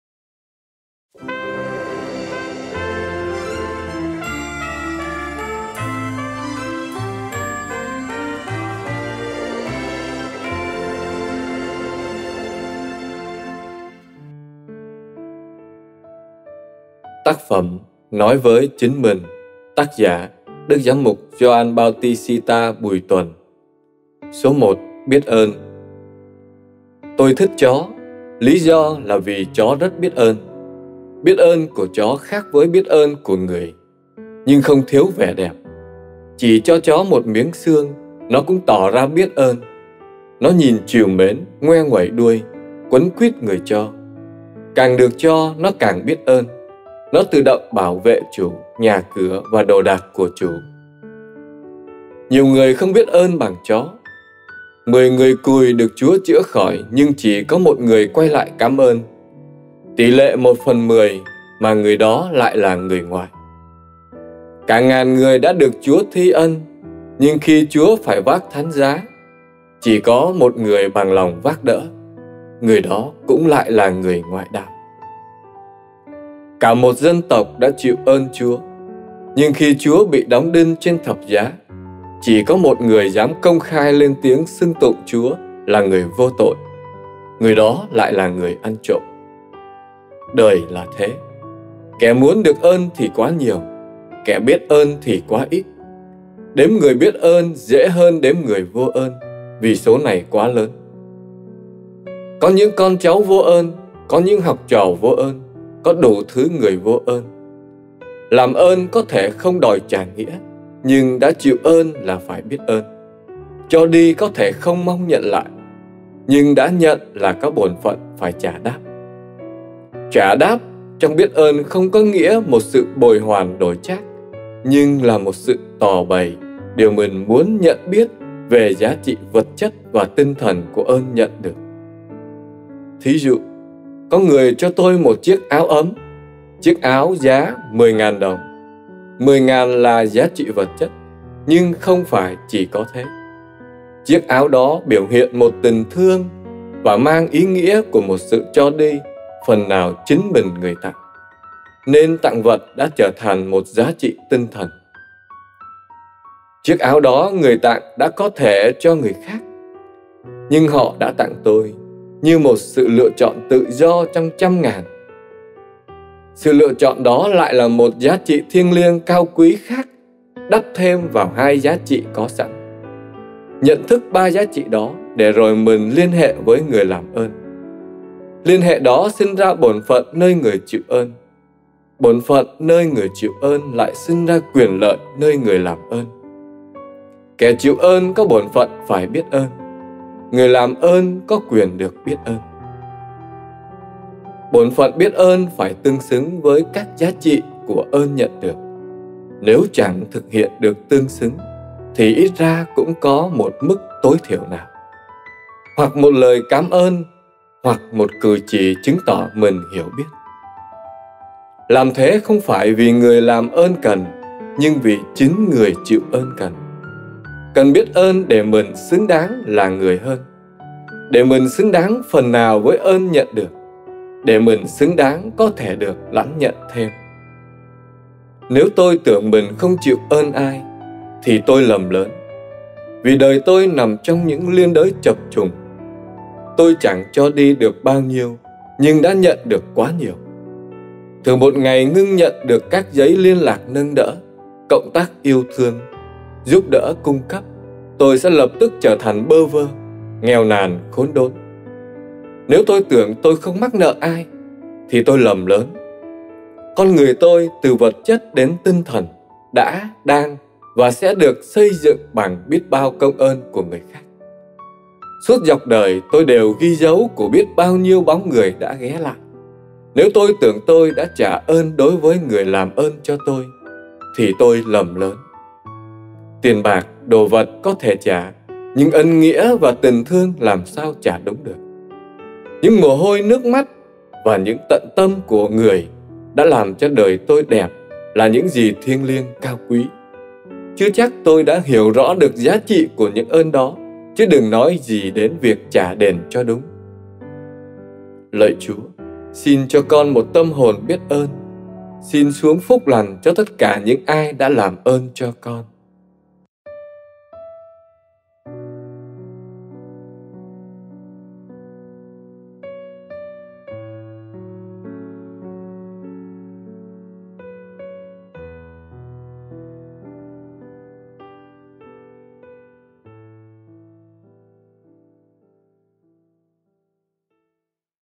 [Nói Với Chính Mình]GB Bùi Tuần - Số 01: Biết Ơn
Nói Với Chính Mình - Số 01: Biết Ơn

Tác giả: Đức Giám Mục Gioan Baotixita Bùi Tuần
Thực hiện: Truyền Thông Giáo Phận Phú Cường
“Nói Với Chính Mình” là một tác phẩm của Đức Giám Mục Gioan Baotixita Bùi Tuần. Được ban truyền thông Giáo phận Phú Cường truyền tải dưới hình thức audio và video với mong muốn tiếp cận gần hơn với quý cộng đoàn.
Xin cảm ơn quý cộng đoàn đã lắng nghe và ủng hộ chúng tôi.